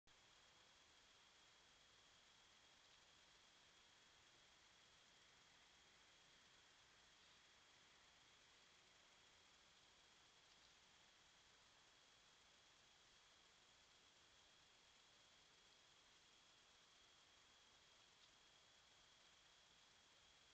Raccoon
Raccoon sniffing around near the water hole.